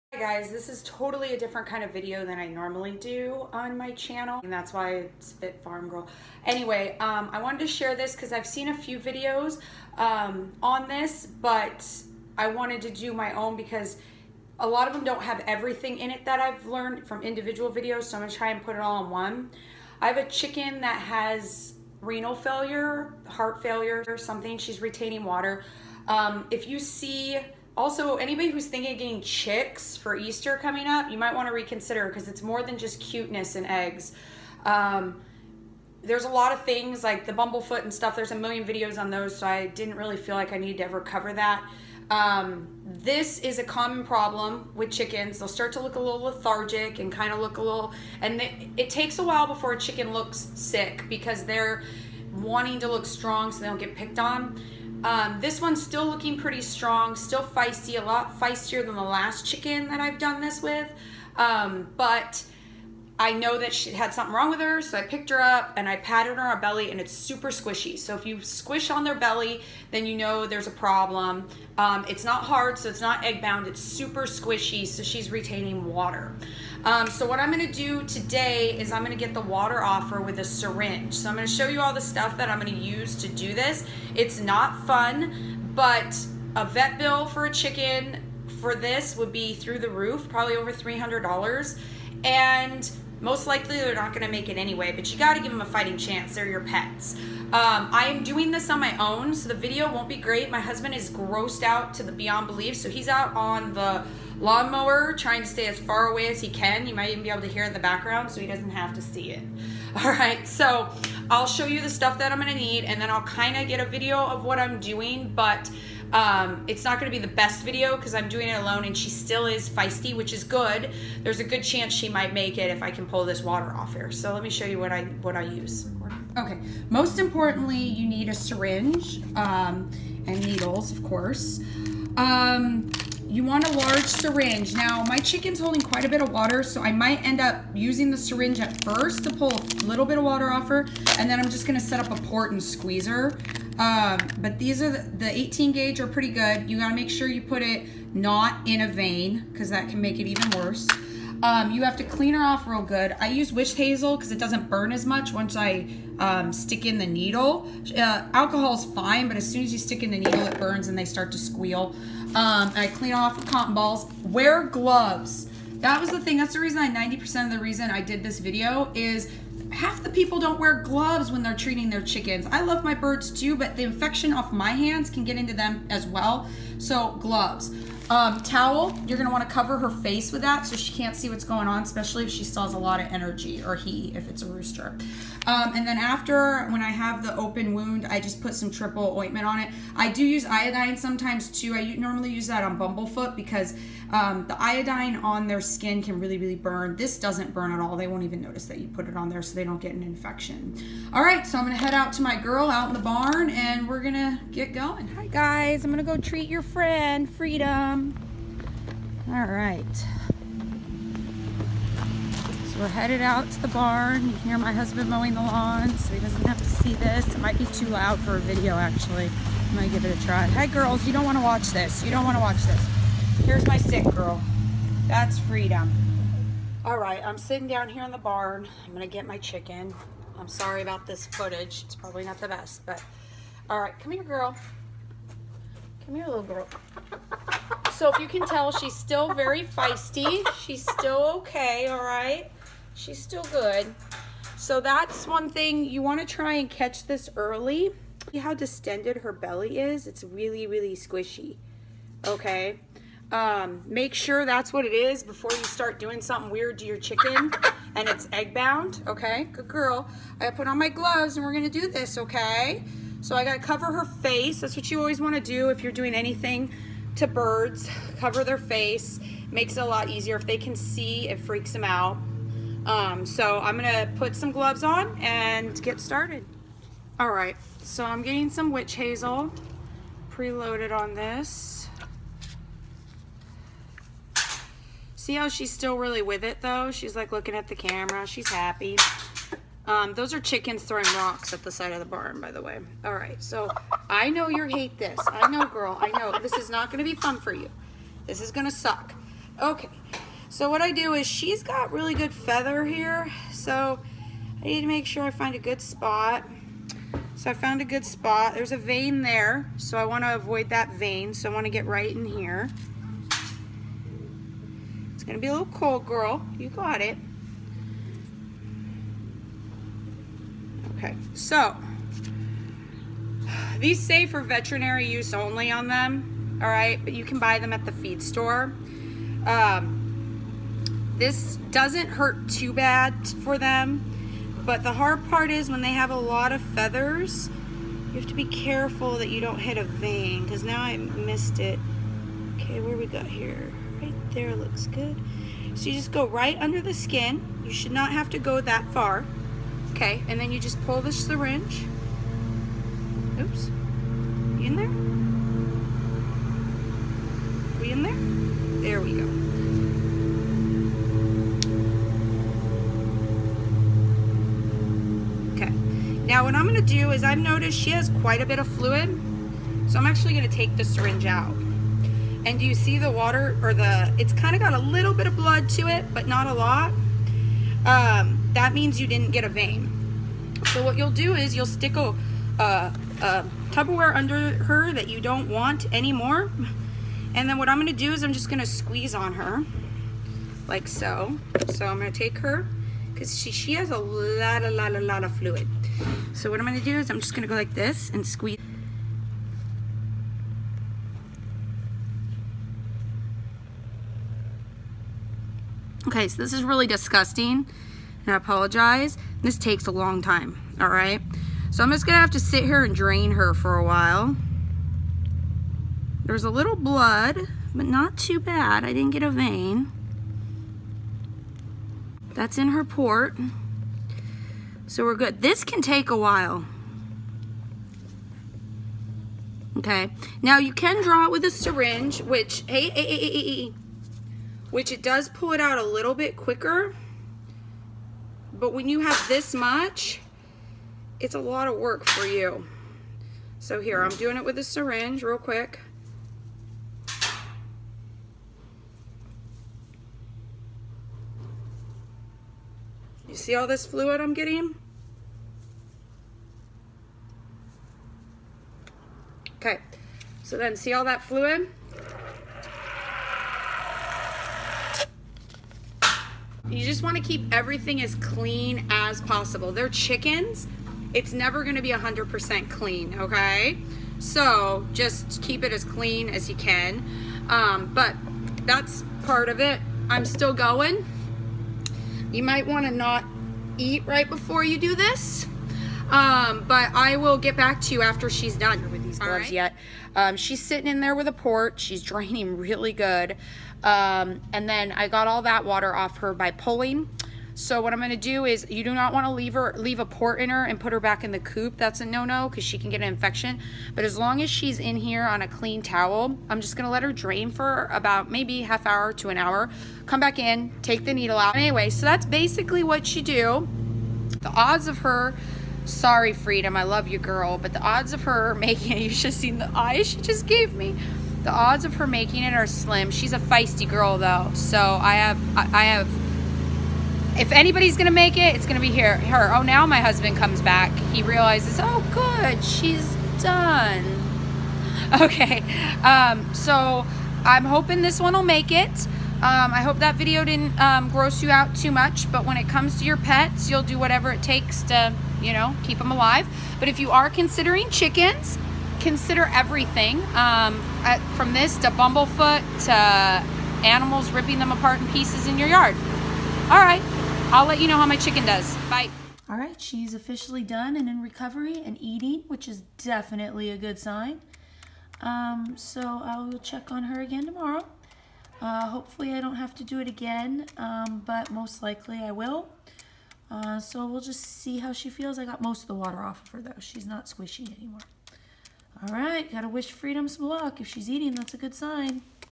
Pulling water off my chicken’s belly with a syringe.
Hello Everyone! I have a 4 year old hen that I noticed was not acting normal. When I picked her up I realized she was retaining a lot of water. It’s probably ascites. Hopefully she didn’t get into any poison. I don’t use any type of poisons on my farm for mice or weeds. But I know others might and with free range chickens it’s hard to know where they go. When you have a lot of chickens like I do at some point you will have to deal with every kind of condition. I’m a self taught vet almost.lol Most of the time if a bird is in renal failure they will probably not make it but I have high hopes for this one!
Mother of chickens try’s to save one of her feather babies lives. 🤞🏻